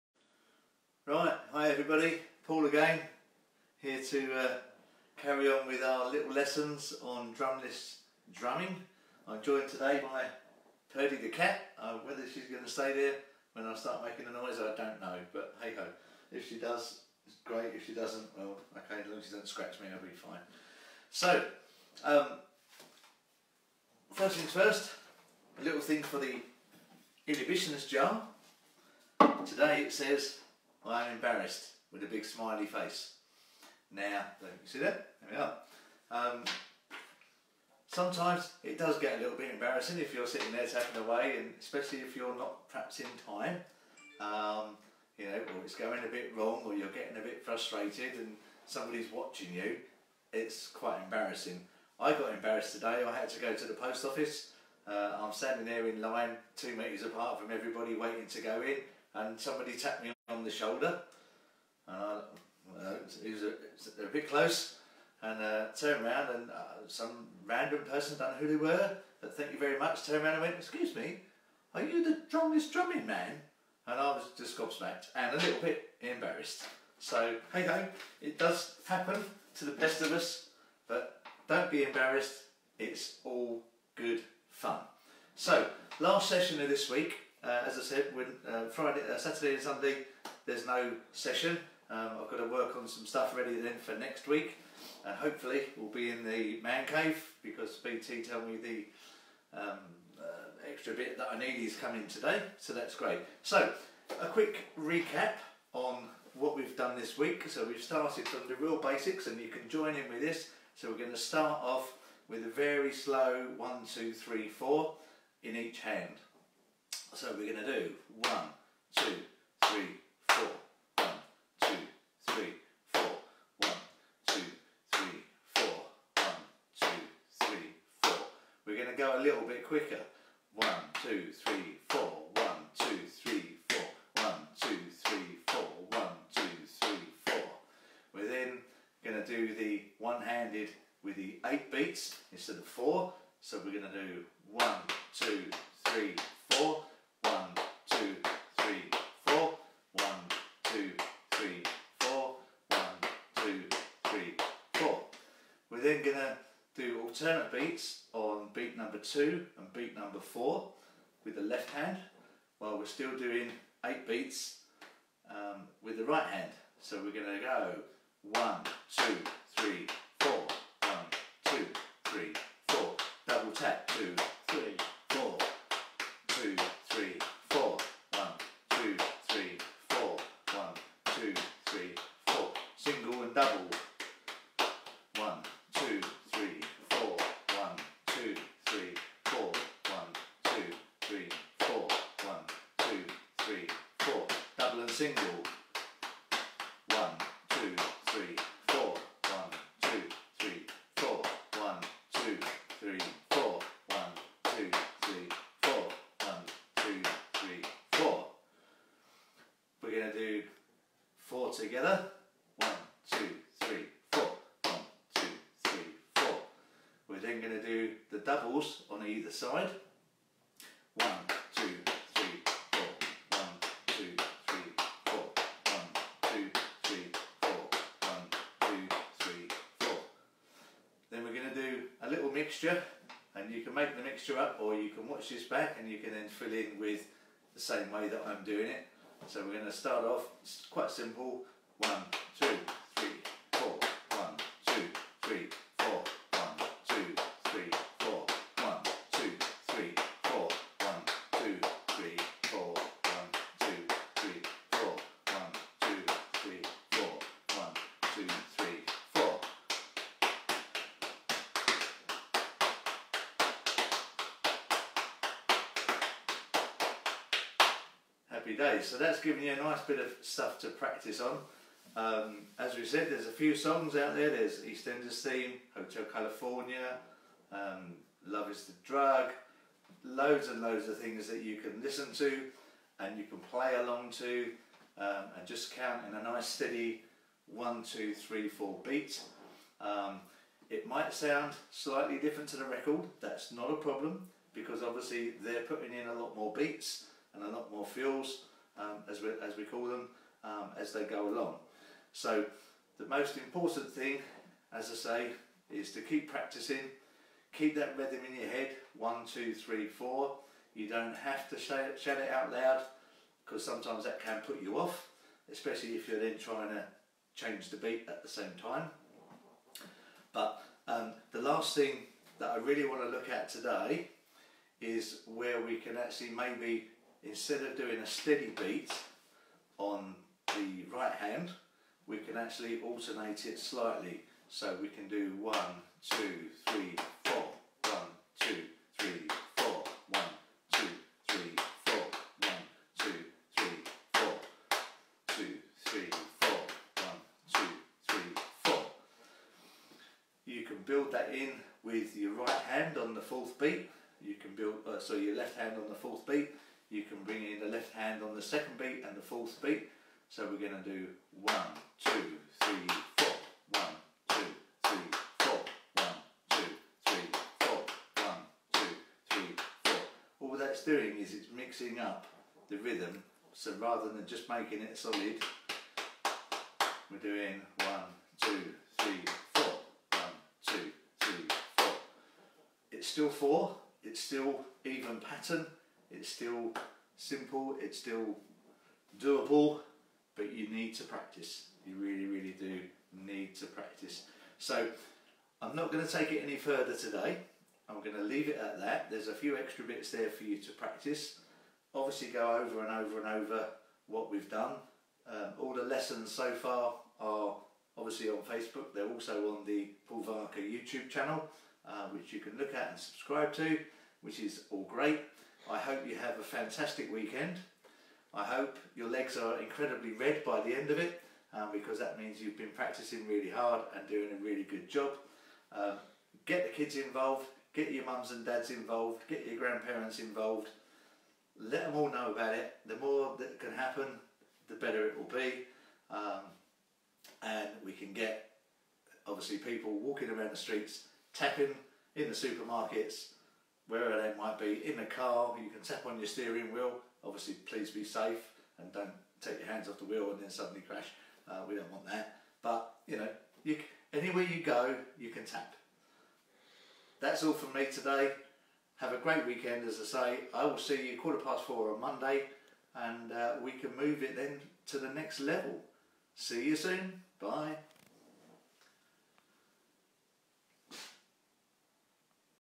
Drumless Drumming for beginners Lesson 5
Learn basic drum beats and rhythms without the need of a drum kit or sticks, suitable for everyone